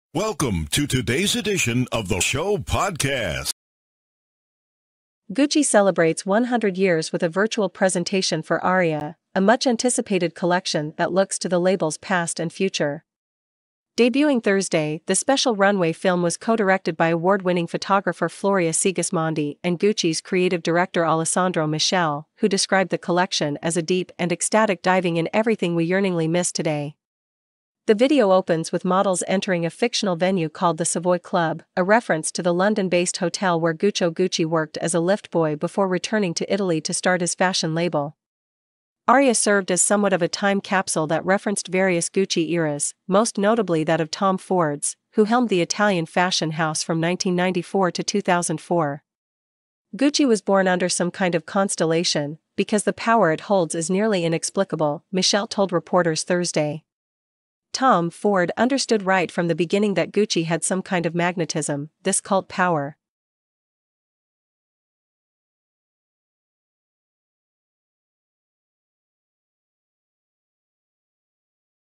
Gucci Presents Its Aria Collection to Soundtrack Featuring Rick Ross, Lil Pump, and More
Gucci celebrates 100 years with a virtual presentation for Aria, a much-anticipated collection that looks to the label’s past and future.

Debuting Thursday, the special runway film was co-directed by award-winning photographer Floria Sigismondi and Gucci’s creative director Alessandro Michele, who described the collection as “a deep and ecstatic diving in everything we yearningly miss today.” The video opens with models entering a fictional venue called the Savoy Club, a reference to the Lon